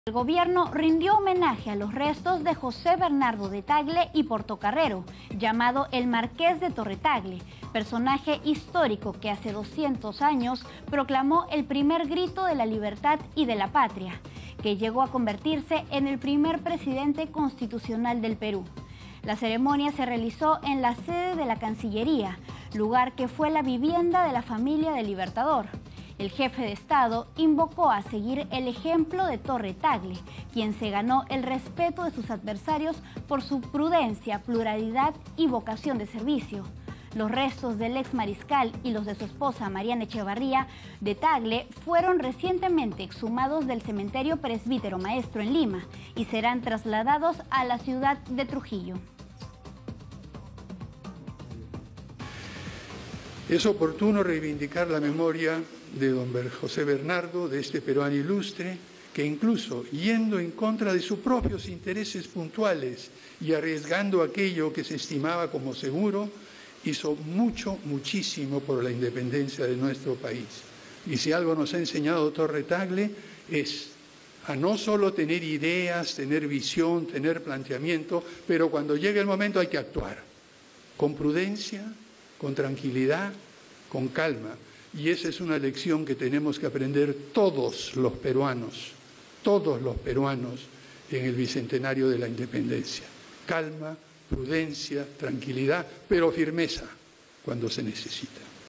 Presidente Sagasti rindió honores a prócer José Bernardo de Tagle
El presidente de la República, Francisco Sagasti, y la ministra de Relaciones Exteriores, Elizabeth Astete, honraron los restos de José Bernardo de Tagle y Portocarrero, Marqués de Torre Tagle, prócer de la Independencia del Perú.

Más información ►